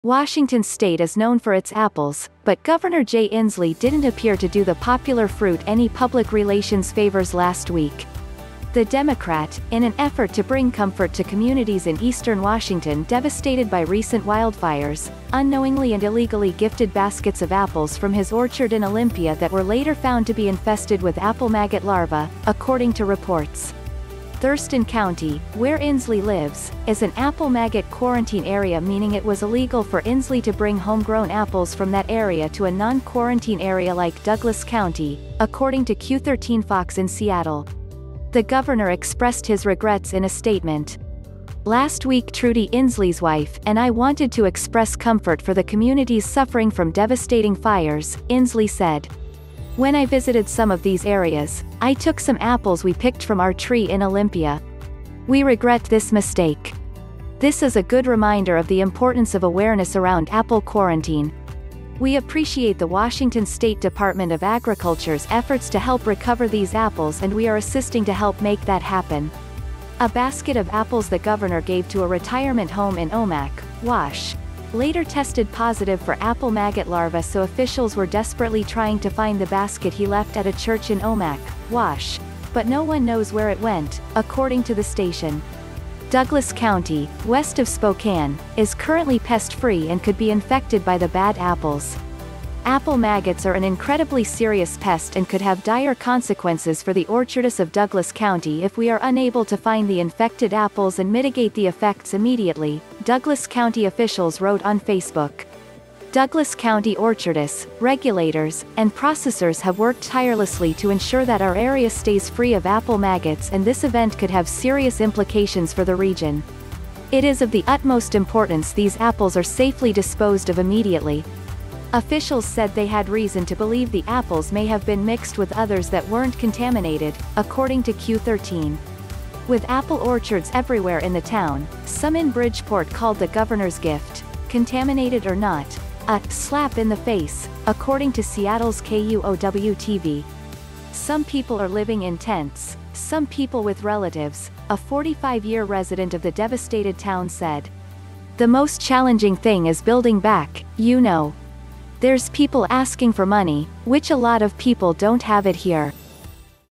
Washington Gov Jay Inslee gives maggot infested apples to wildfire
Washington state is known for its apples — but Gov. Jay Inslee didn’t appear to do the popular fruit any public-relations favors last week. The Democrat, in an effort to bring comfort to communities in Eastern Washington devastated by recent wildfires, unknowingly and illegally gifted baskets of apples from his orchard in Olympia that were later found to be infested with apple maggot larvae, according to reports. Thurston County, where Inslee lives, is an apple maggot quarantine area –meaning it was illegal for Inslee to bring homegrown apples from that area to a non-quarantine area like Douglas County, according to Q13 FOX in Seattle. The governor expressed his regrets in a statement.“Last week Trudi [Inslee’s wife] and I wanted to express comfort for the communities suffering from devastating fires, ” Inslee said. “When I visited some of these areas, I took some apples we picked from our tree in Olympia. We regret this mistake. This is a good reminder of the importance of awareness around apple quarantine. We appreciate the Washington State Department of Agriculture’s efforts to help recover these apples and we are assisting to help make that happen. ”A basket of apples the governor gave to a retirement home in Omak, Wash., later tested positive for apple maggot larvae so officials were desperately trying to find the basket he left at a church in Omak, Wash., but no one knows where it went, according to the station. Douglas County, west of Spokane, is currently pest-free and could be infected by the bad apples.“Apple Maggots are an incredibly serious pest and could have dire consequences for the orchardists of Douglas County if we are unable to find the infected apples and mitigate the effects immediately, ” Douglas County officials wrote on Facebook. “Douglas County orchardists, regulators, and processors have worked tirelessly to ensure that our area stays free of apple maggots and this event could have serious implications for the region. It is of the utmost importance these apples are safely disposed of immediately. ”Officials said they had reason to believe the apples may have been mixed with others that weren’t contaminated, according to Q13. With apple orchards everywhere in the town, some in Bridgeport called the governor’s gift – contaminated or not — a “slap in the face, ” according to Seattle’s KUOW-TV.“Some people are living in tents; some people with relatives, ” a 45-year resident of the devastated town said. “The most challenging thing is building back, you know. There’s people asking for money, which a lot of people don’t have it here. ”